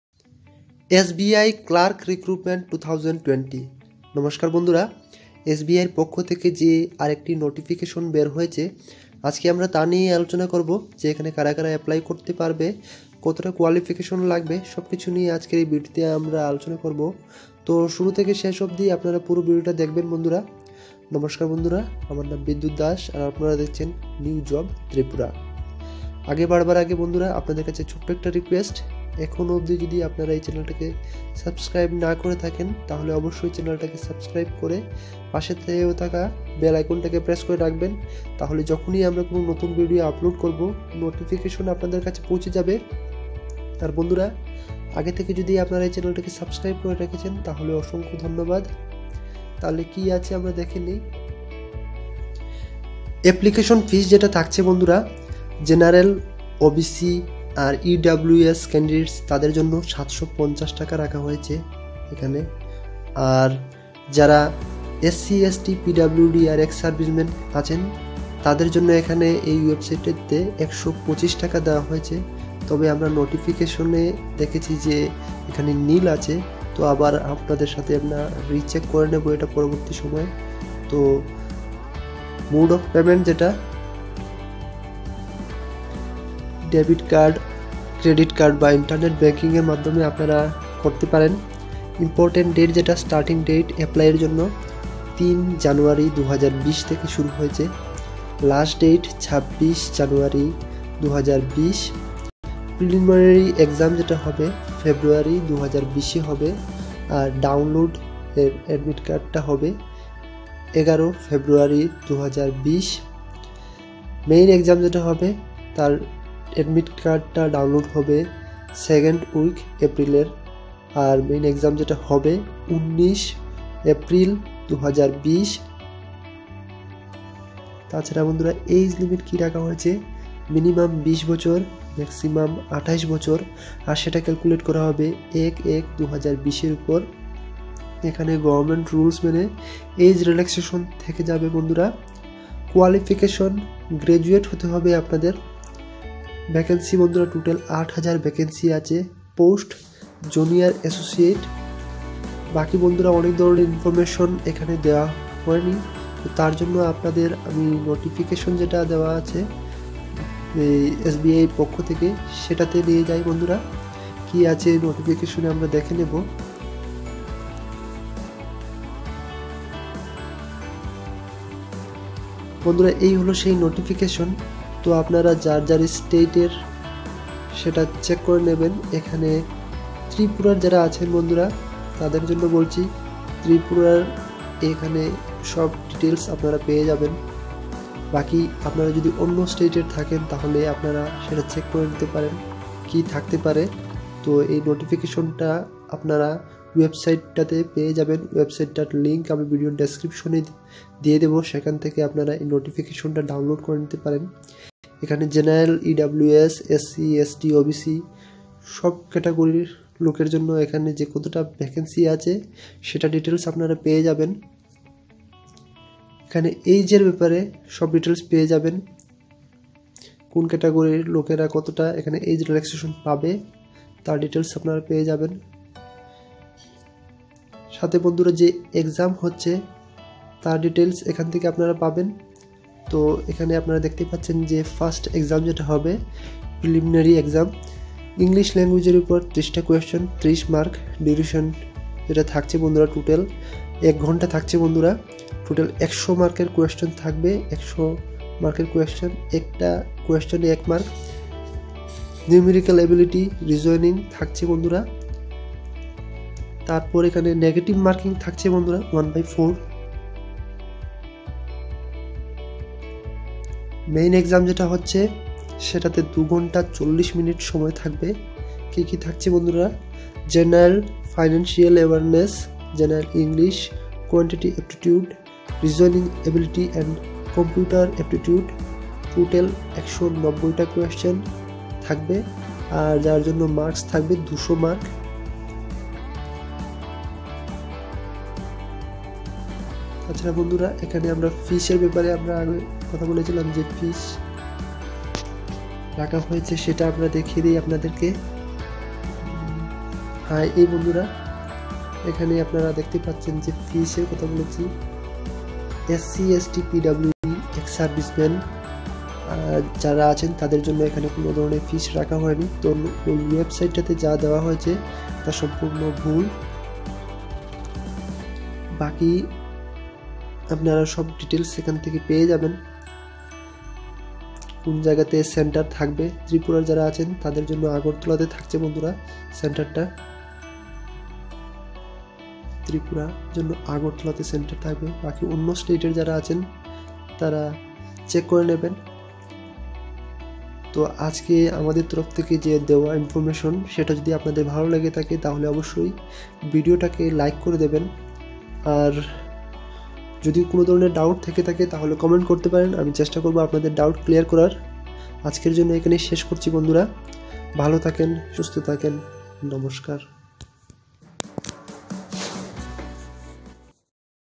Tripura Latest Govt Job News 2020 In Bangla Today | New Job Tripura
SBIClerkRecruitment2020 | SBI Apply Online for 8134 Posts

State Bank of India (SBI) has given an employment notification for the recruitment of Junior Associate (Customer Support & Sales) in Clerical Cadre (Regular & Backlog) vacancies. Those Candidates who are Interested to the following vacancy and completed all Eligibility Criteria can read the notification & Apply online.

Clerk Vacancies 2020
Advt No. CRPD/CR/2019-20/20

 Application Fee

For General/ OBC/ EWS: Rs. 750/- (Including Intimation Charges)

For SC/ST/PWD/XS: Rs. NILL (Intimation Charges Only)

Mode of Payment: Debit card/ Credit card/ Internet Banking

Important Dates

Starting Date for Apply Online & Payment of Fee: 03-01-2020

Last Date to Apply Online & Payment of Fee: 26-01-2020

Last Date for Editing Application Details: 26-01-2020

Last Date for Printing Your Application: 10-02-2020

Date of Preliminary Examination (Tentative): February/ March 2020

Date of Downloading Admit Cards (Prelims Exam): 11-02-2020 (Tentatively)

Date of Downloading Admit Cards (Main Exam): 2nd Week of April 2020 (Tentatively)

Date of Main Examination (Tentative): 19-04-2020

Minimum: 20 years

Maximum: 28 years

Age Relaxation is applicable as per rules.

Qualification

Graduation in any discipline.

tripura latest govt jobs 2020,
tripura latest news,
tripura latest news today in bengla,
tripura latest news today,